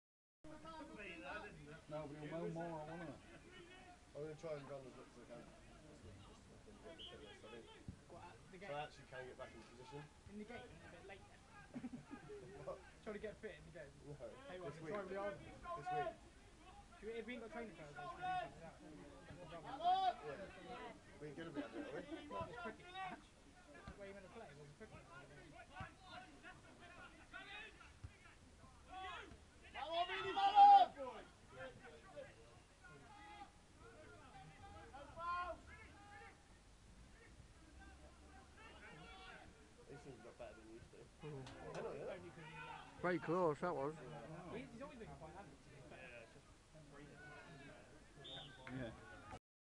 EAST CORNWALL PREMIER LEAGUE GAME, St Stephen 1st 1 v 1 Veryan
Match highlights, St Stephen player Paul Reed goes on a run but eventually gets fouled.